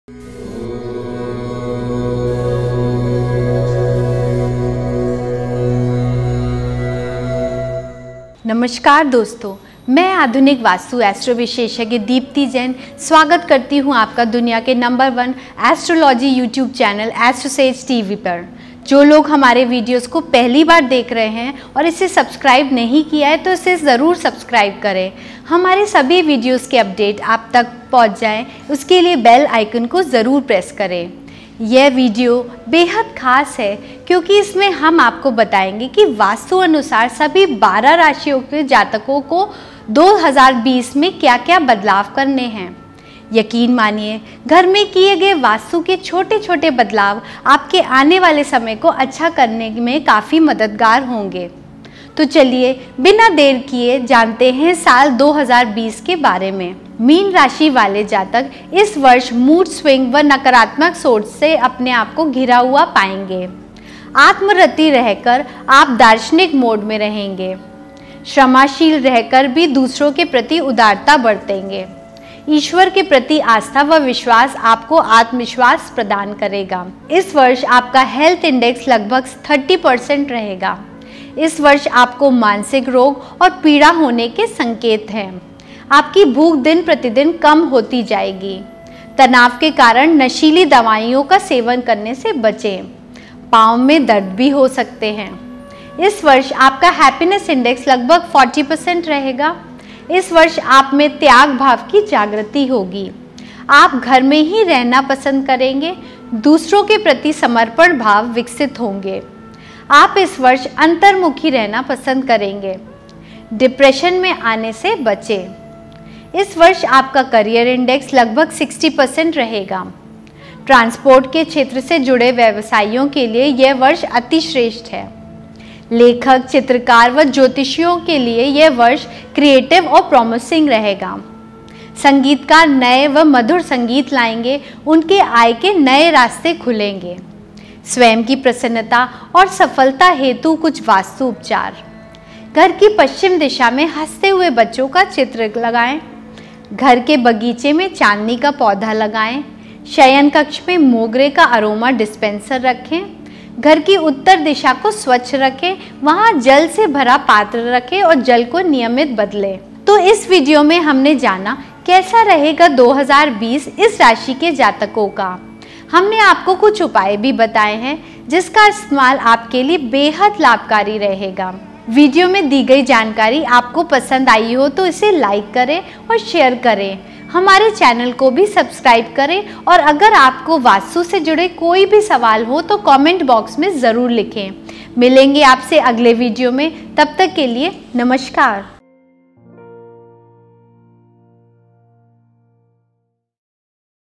Meen Rashifal 2020 Based On Vastu - मीन राशिफल 2020 वास्तु के आधार पर
This Meen Rashifal 2020 is based on the principles of Vastu Shastra. The Pisces 2020 horoscope in Hindi as per Vastu will give you predictions on how this year is going to be for you along with remedies based on Vaastu. The remedies given here cover a wide variety of areas such as office vastu, home vastu, kitchen vastu, factory vastu, and much more. These Vastu remedies will help you avoid potholes in 2020 and have a prosperous and successful life.

यह मीन राशिफल 2020 वास्तु शास्त्र के सिद्धांतों पर आधारित है। वास्तु आधारित वर्ष 2020 का यह फलादेश आपको बताएगा कि यह साल आपके लिए कैसा रहेगा, कौन-कौन सी समस्याएँ आपके सामने आ सकती हैं और वास्तु के अनुसार उन्हें दूर करने का क्या उपाय है। यहाँ दिए गए उपायों में वास्तु के विभिन्न उपयोग यथा गृह वास्तु, ऑफिस वास्तु, किचन वास्तु, फेक्ट्री वास्तु आदि सम्मिलित हैं। इस 2020 के मीन राशिफल और इन वास्तु उपायों के द्वारा आप संभावित परेशानियों से निजात पा सकते हैं और 2020 को समृद्ध व सुखद बना सकते हैं।

For Personalized services & inquiries E- Mail